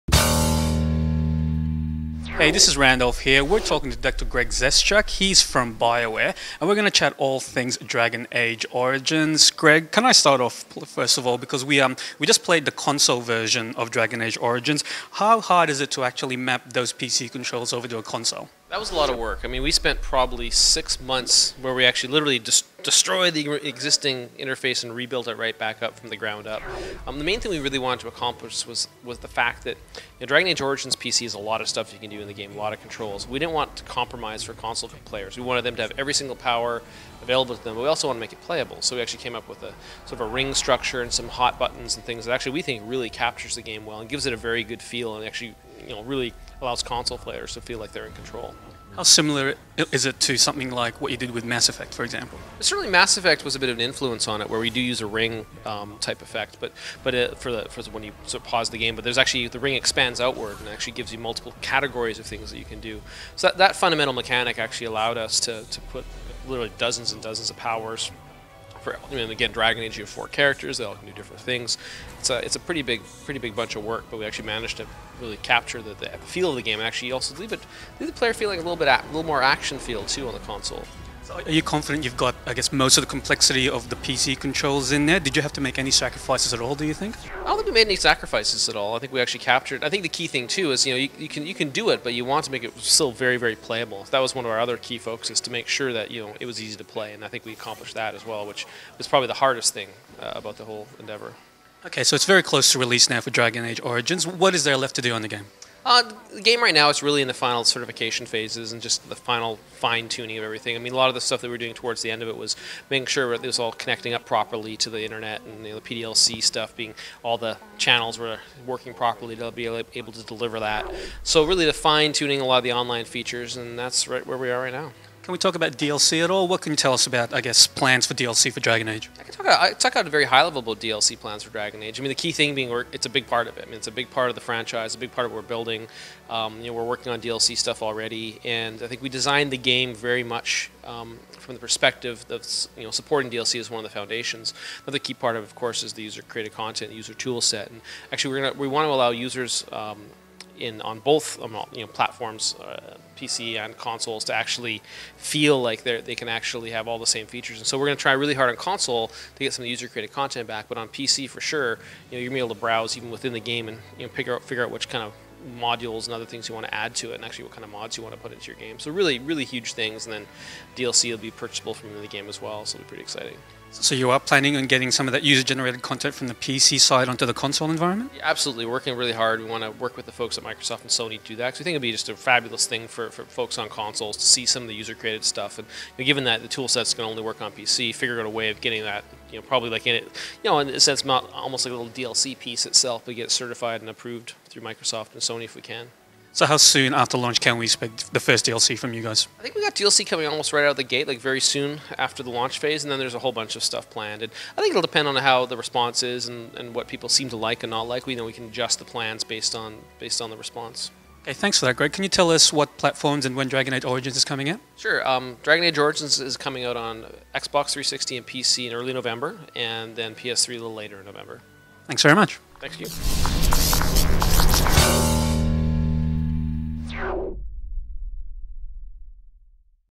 Dragon Age Origins Interview with Dr. Greg Zeschuk
Randolph Ramsay chats to Dr. Greg Zeschuk of Bioware about Dragon Age: Origins' console controls, DLC, and user-created content.